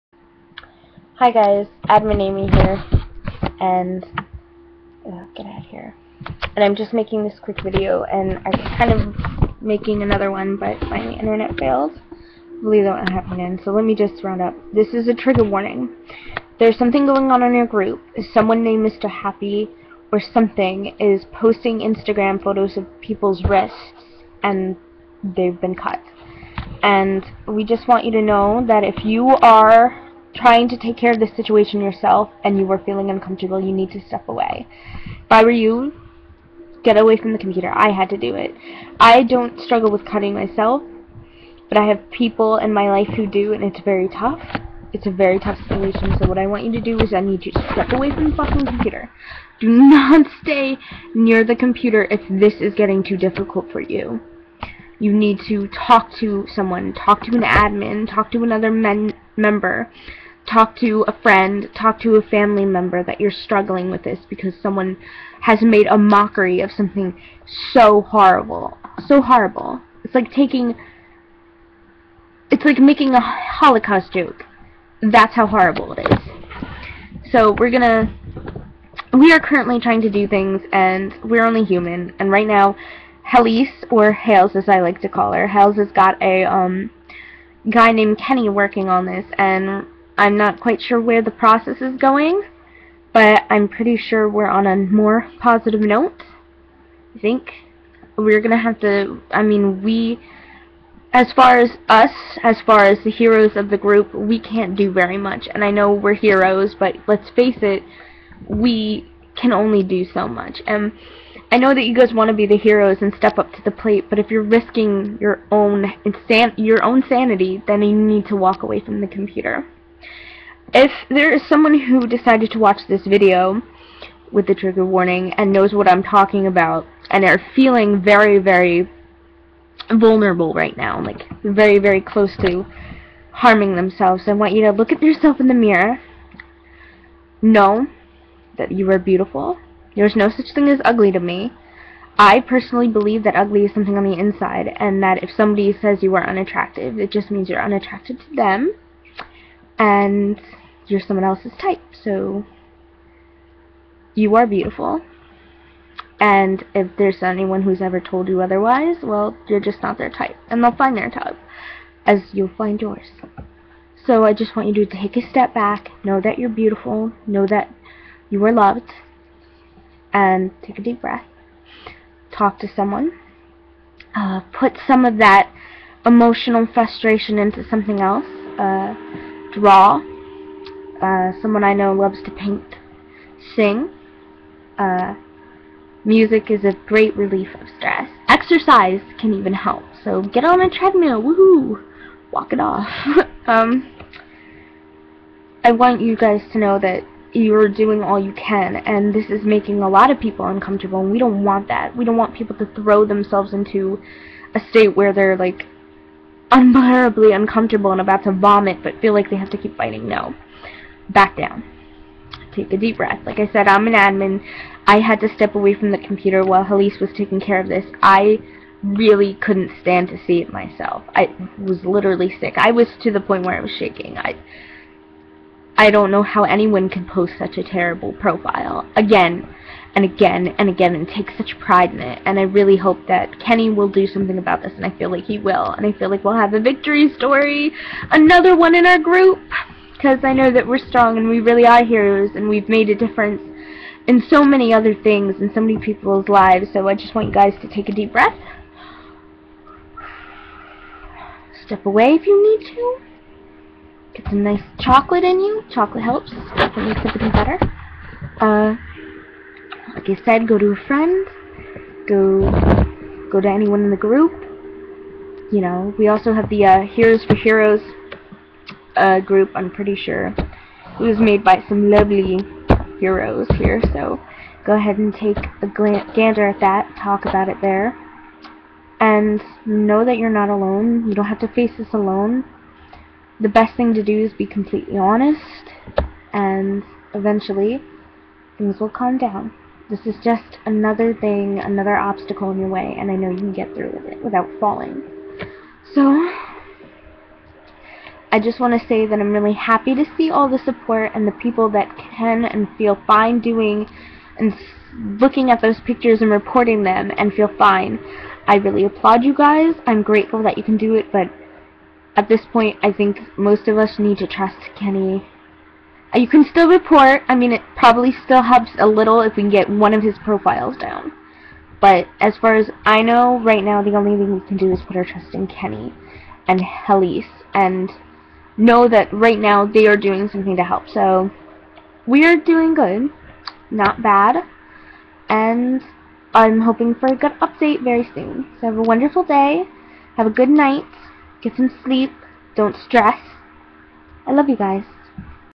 Trigger Warning!!! Mr.Happy my fat white ass!!!
This has a lot of detail about the profile on Mr. Happy. Please read carefully.

TRIGGER WARNING: THIS DOES MENTION SELF-HARM! PLEASE IGNORE IF YOU ARE ALREADY STUGGLING!

THIS VIDEO MENTIONS MR.HAPPY AND WHAT WE ARE DOING TO HELP. REMEMBER IF THIS MAKES YOU UNHAPPY, STRESS, UNCOMFORTABLE, PLEASE WALK AWAY FROM THE COMPUTER. IF THIS POST HAS CAUSED YOU TO WANT TO SELF HARM SEEK A FRIEND WITHIN THE GROUP! THANK YOU!

ADMIN AMY ~HELLO, BABY!~